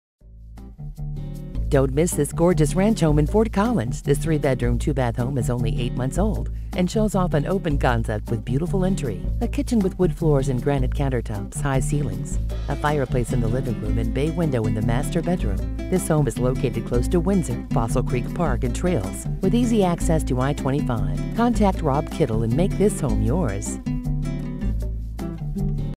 Home for sale in Fort Collins, CO | $318,000
Gorgeous ranch home-only 8 mos old w/transferable 24 month bldr warranty. Beautiful entry! Open concept with high ceilings. Kitch features granite countertops, tile backsplash, wood floors, 42&quot; alder cabinets w/crown moulding and pantry. Window seat in Din Rm. Fireplace in Liv Rm. Bay window in Mstr Bdrm. Decorator tiles in bath &amp; kitch. Fin tandem 3-car garage w/epoxy floor, insulated &amp; built-in cabinets. Close to Lvld &amp; Windsor. Fossil Creek Park &amp; trails close by. Easy access to I-25.